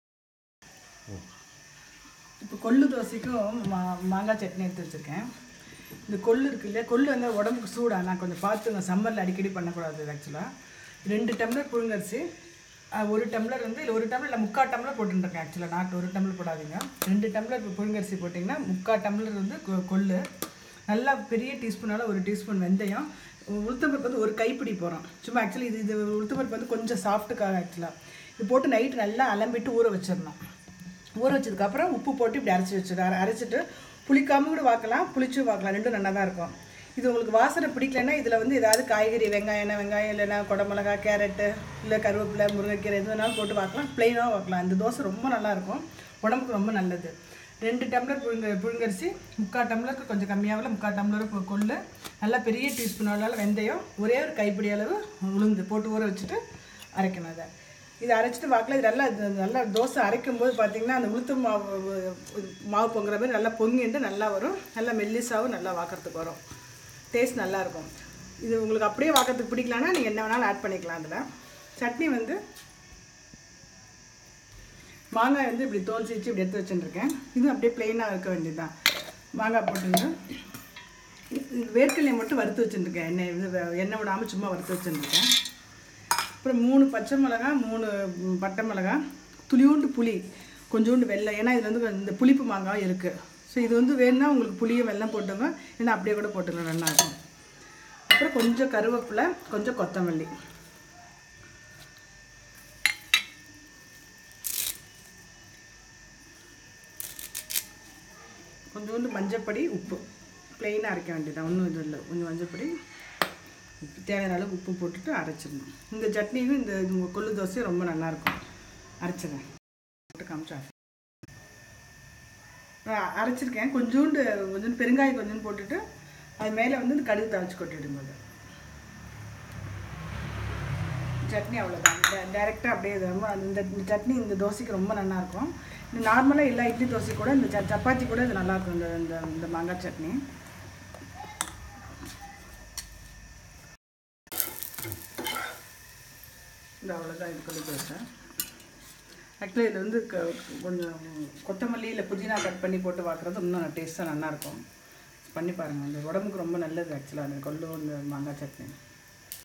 கொள்ளு தோசை மாங்காய் சட்னி Horse Gram Dosa, Mango Chutney
Ingredients

Boiled Rice - 500gms
Kollu - 200gms
Urad Dhal - 10 gms
Fenugreek - 5 gms

For Chutney

Mango - 1/2
Ground Nut - 25gms roasted
Tamarind - marble size
Jaggary - little - Optional
Curry Leaves - little
Corriander leaves - little
Asafoetida
Mustard
Salt